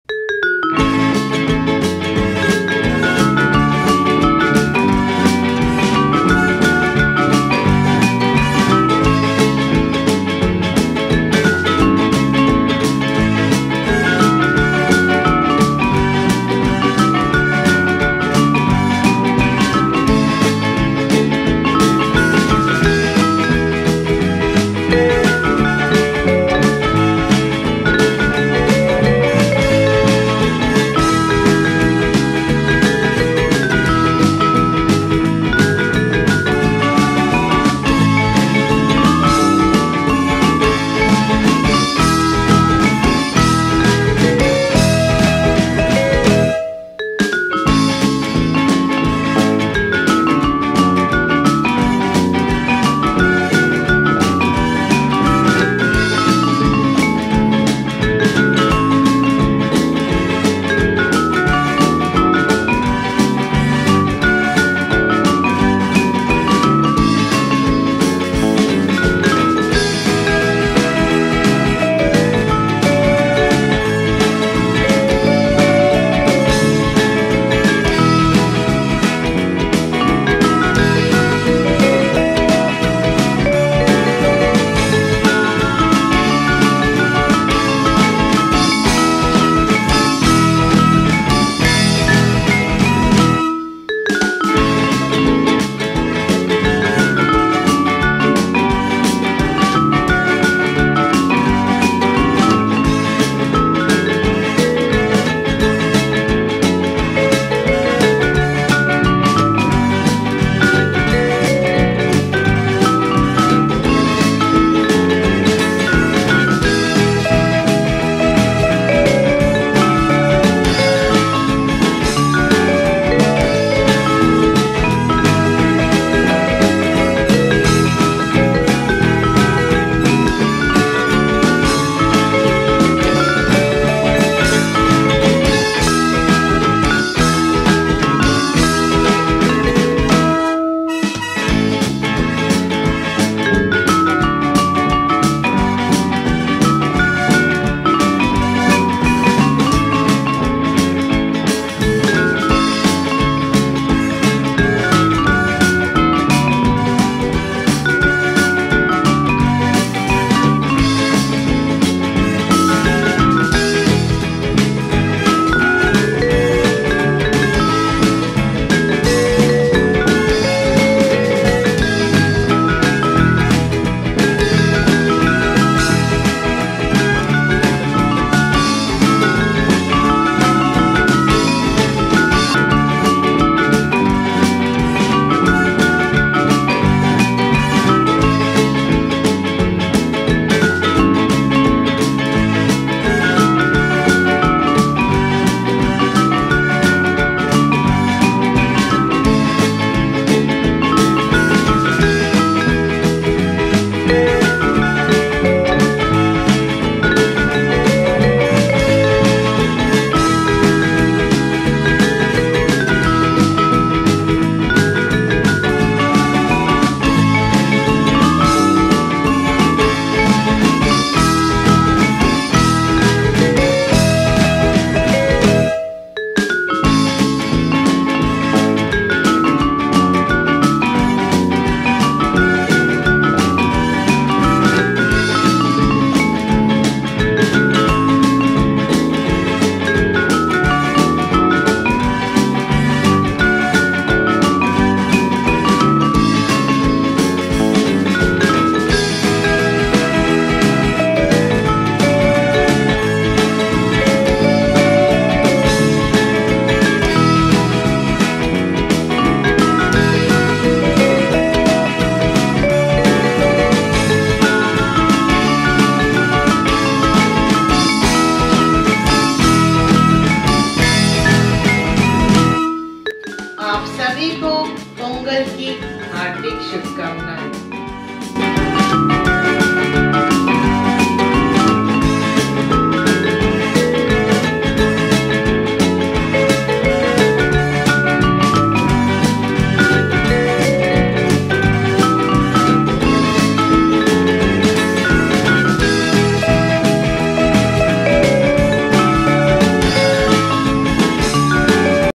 Pongal Decoration at Home 2024
Pongal, also referred to as Thai Pongal, is a multi-day Hindu harvest festival celebrated by Tamils. It is observed in the month of Thai according to the Tamil solar calendar and usually falls on 14 or 15 January.